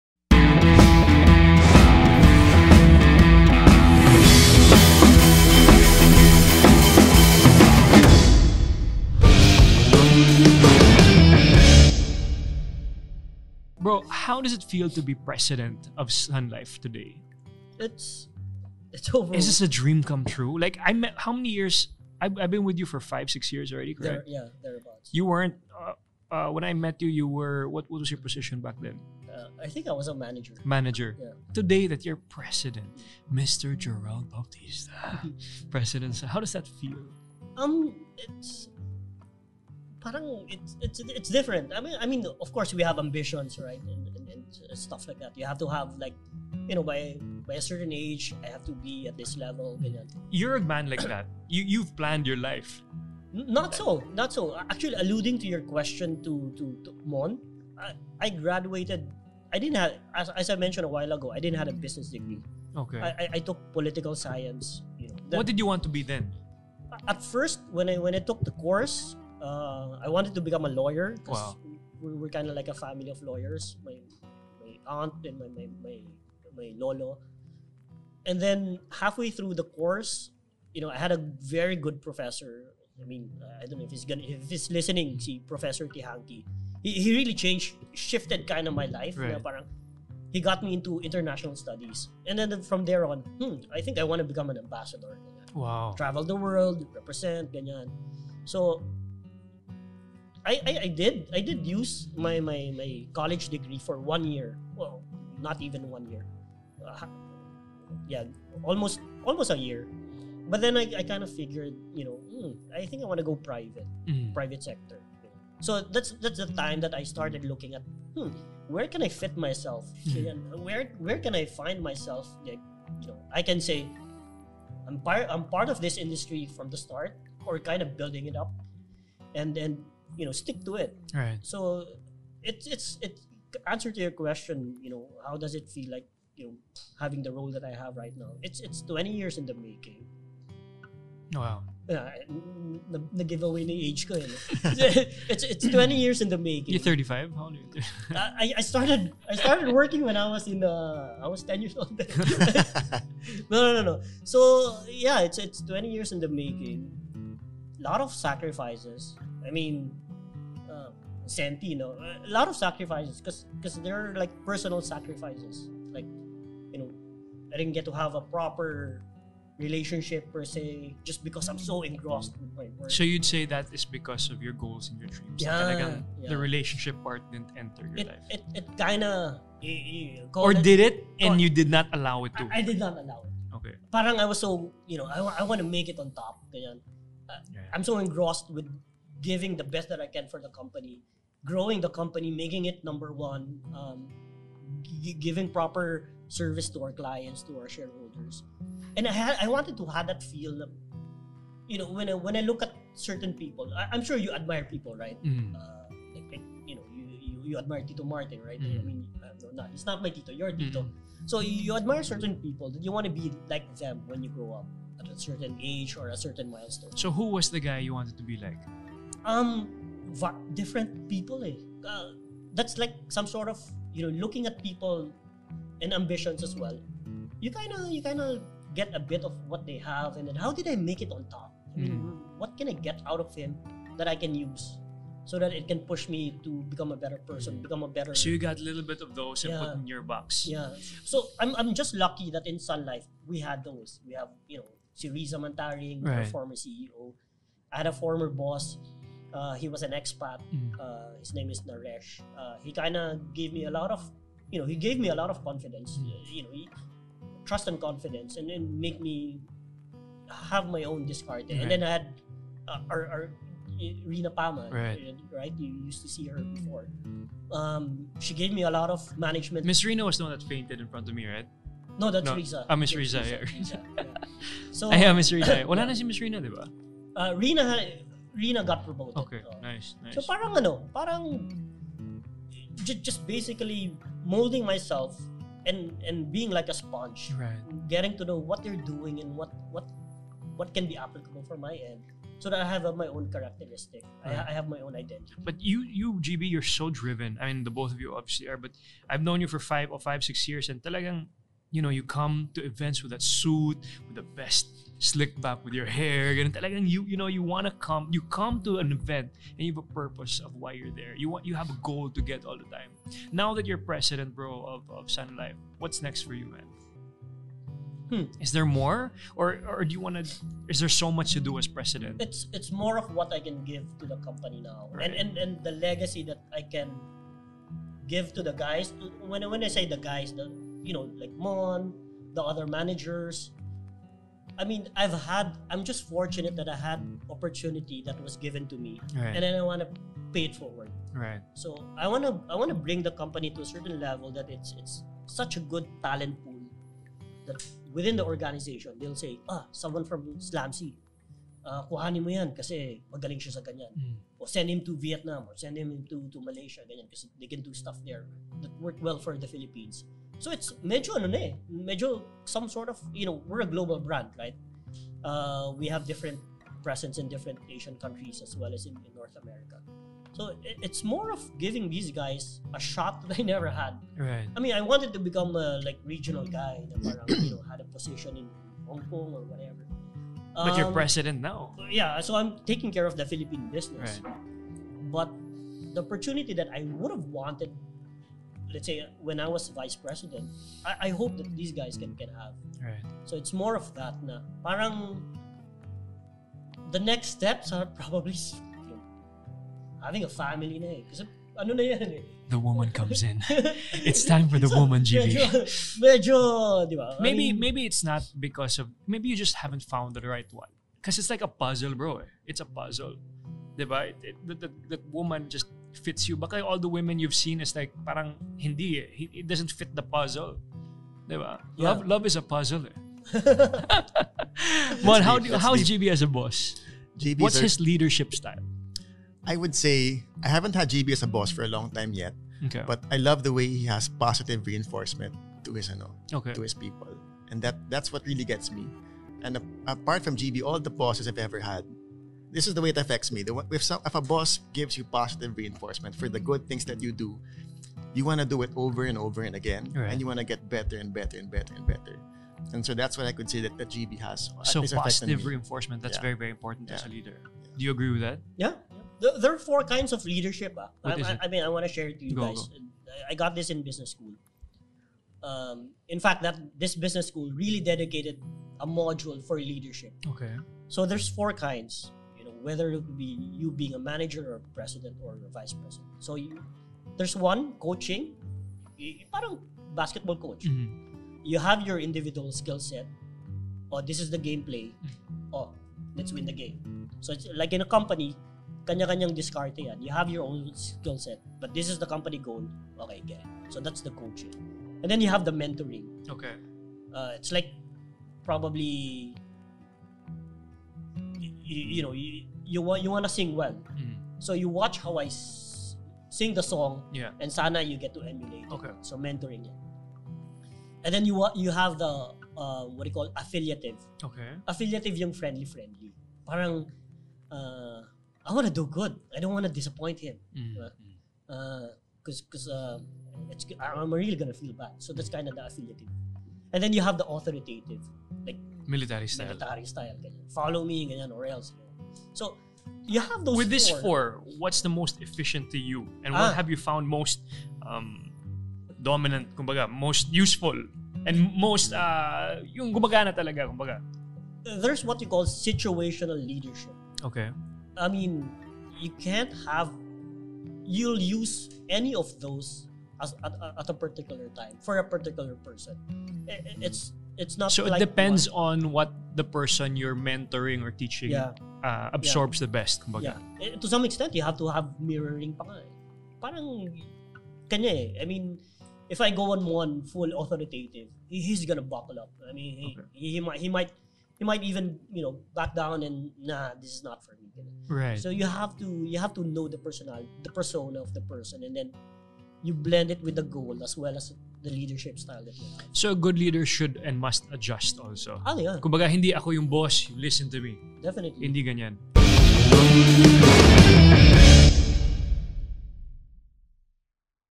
What Makes A Good Leader | Gerald Bautista & Mon Osmeña
Sun Life Asset Management President Gerald Bautista shares how admiring the right people led him to achieving his goals and what for him makes a good leader.

Let's keep the conversation running! Listen to MattRuns only on Spotify and Apple Podcasts! (spoti.fi/3x5nZ4S)